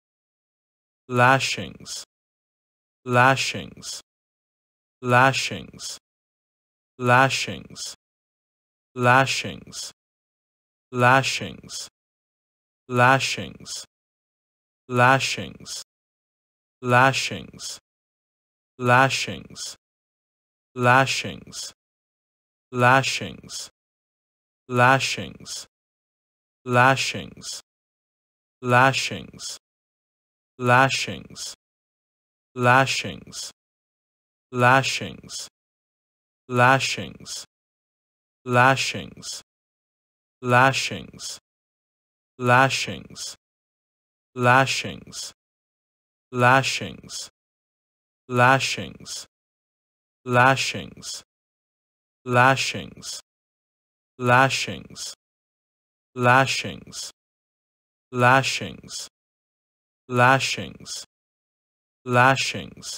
How to pronounce Lashings||How to say Lashings||Lashings Pronunciation||ABDictionary
Do you want to know how to pronounce Lashings? In this video you'll learn how to say or pronounce Lashings for free